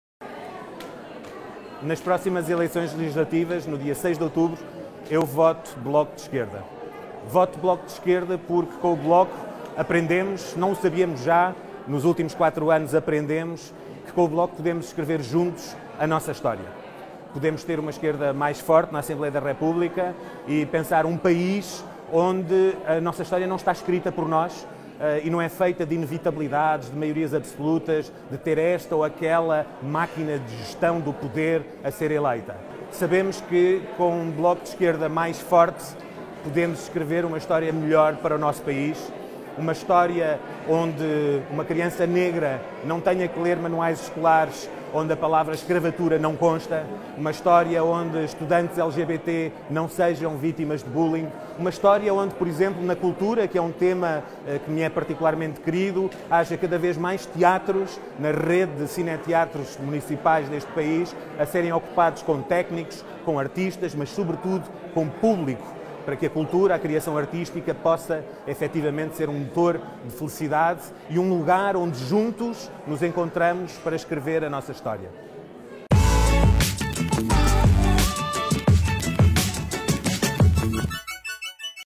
Tiago Rodrigues declara o seu apoio ao Bloco | ESQUERDA.NET
Tiago Rodrigues, Diretor do Teatro Nacional D. Maria II, declara publicamente o seu apoio ao Bloco de Esquerda para as legislativas19 de Domingo.